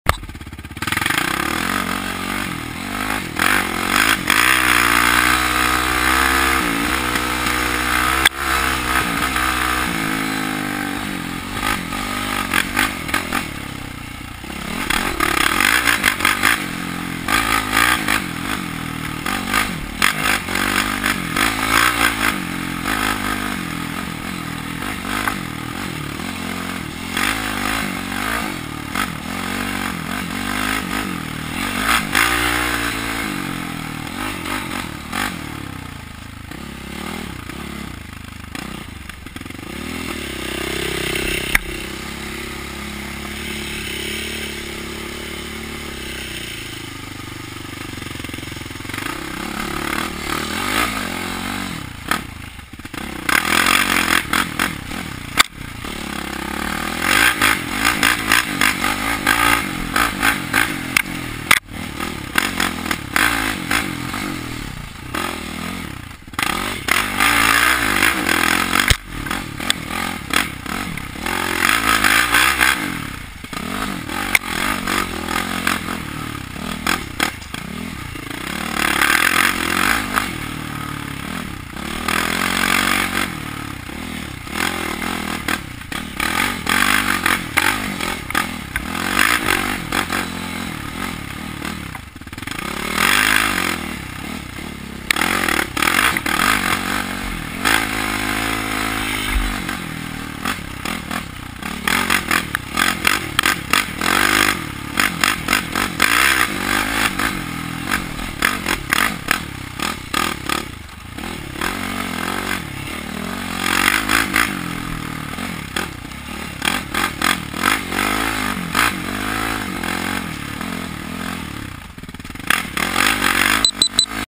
Creek trail
riding thru my creek trail trying to get my sliding turns down.